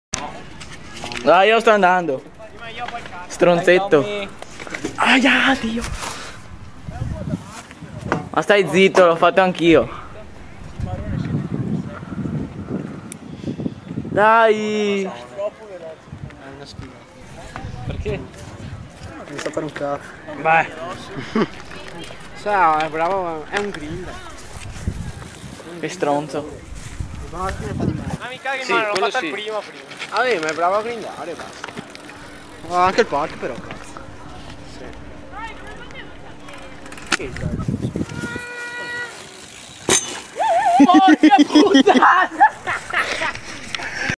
Saltone da Salmone e da Vitellone Tonnato with my bike looool
io ke salto giù da un droppone e rimbalzo, e croccolax, il mio amico ke nn sa ke vitelli tonnare si mette a ridere come un defficiente haha loool